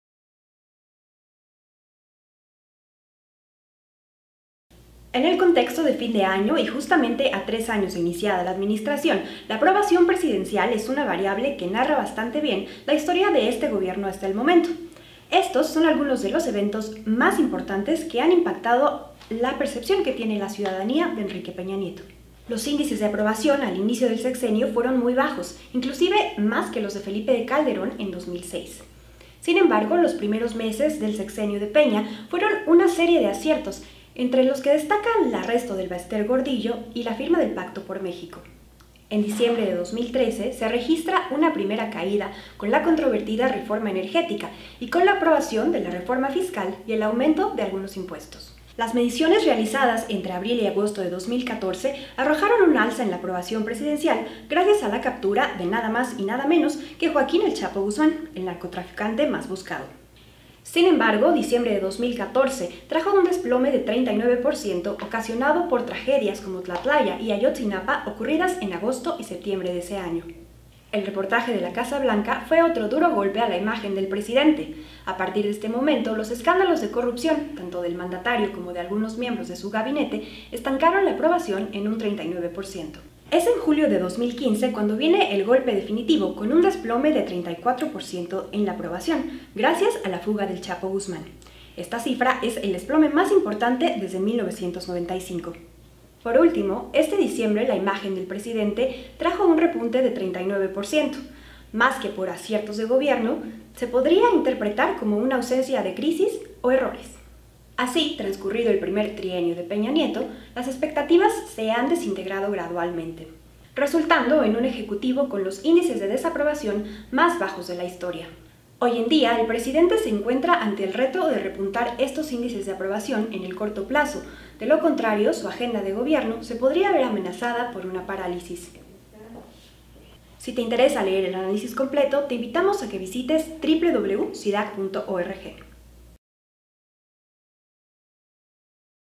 ¿Cómo lo ha hecho Peña Nieto?
En 2012 Enrique Peña Nieto llegó a la presidencia con el lema “te lo firmo y te lo cumplo”. Transcurrido el primer trienio de Peña, dichas expectativas se han desintegrado gradualmente.